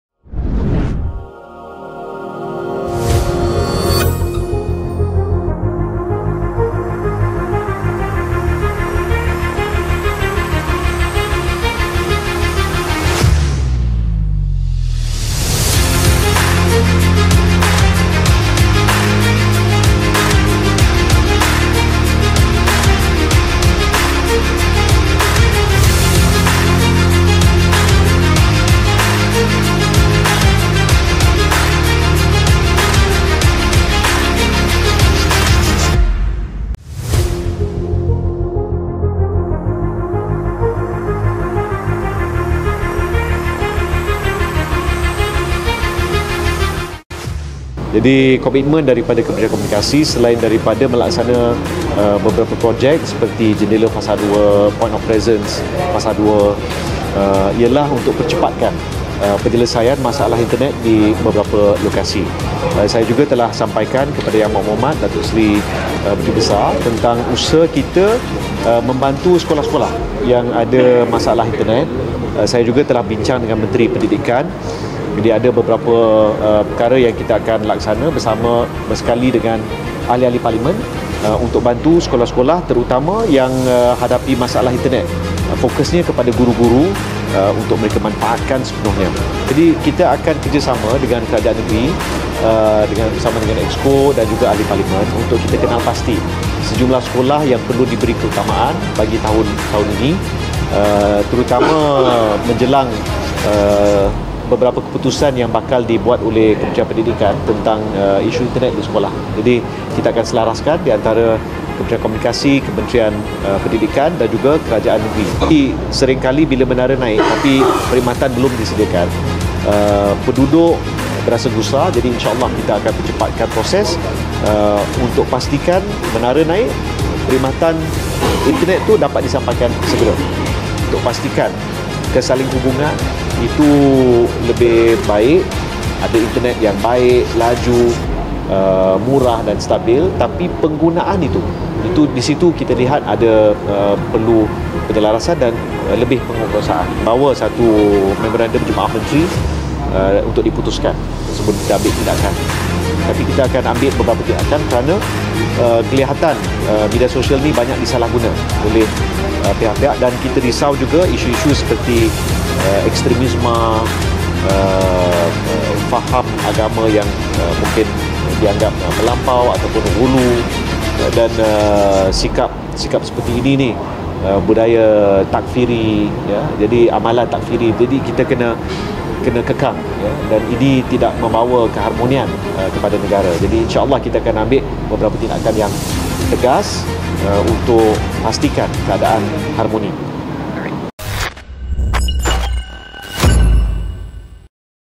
Kunjungan Hormat ke atas YAB Dato’ Sri Haji Wan Rosdy Wan Ismail, Menteri Besar Pahang
|Sorotan Video| KUANTAN, 2 FEBRUARI 2024 : Menteri Komunikasi Fahmi Fadzil, memulakan lawatan kerja di Pahang dengan mengadakan kunjungan hormat ke atas Menteri Besar Pahang Dato' Sri Haji Wan Rosdy Wan Ismail di Wisma Sri Pahang.

Turut mengiringi beliau Timbalan Menteri Komunikasi Teo Nie Ching, Ketua Setiausaha Kementerian Komunikasi Datuk Mohamad Fauzi Md Isa, Ketua Pengarah Penyiaran Datuk Suhaimi Sulaiman, Ketua Pengarah Jabatan Penerangan Malaysia Datuk Mohd Sukari Ab Hamid, Pengarah Jabatan Penerangan Negeri Pahang En. Mazli Bin Haji Hamdan dan Pengerusi Suruhanjaya Komunikasi dan Multimedia Bukhari Yahya.

Menurut Fahmi, pelbagai isu telah dibincangkan yang akan memberi manfaat kepada rakyat di negeri ini. “Antaranya isu melibatkan talian internet dan talian telefon yang menjadi keperluan utama di sekolah-sekolah dan kawasan pedalaman khususnya kawasan penempatan orang asli,” katanya.

Kunjungan hormat ini disambut baik oleh Menteri Besar Pahang dan beliau berharap semoga dengan lawatan ini hubungan antara Kerajaan Negeri Pahang dengan Kementerian Komunikasi akan lebih kukuh.

Turut hadir Exco Komunikasi dan Multimedia, Belia, Sukan dan Badan-badan Bukan Kerajaan Negeri Pahang, Fadzli Mohamad Kamal.

Video  : Azhan Mahayuddin/Izzat Abd Rahman
Editor  : Izzat Abd Rahman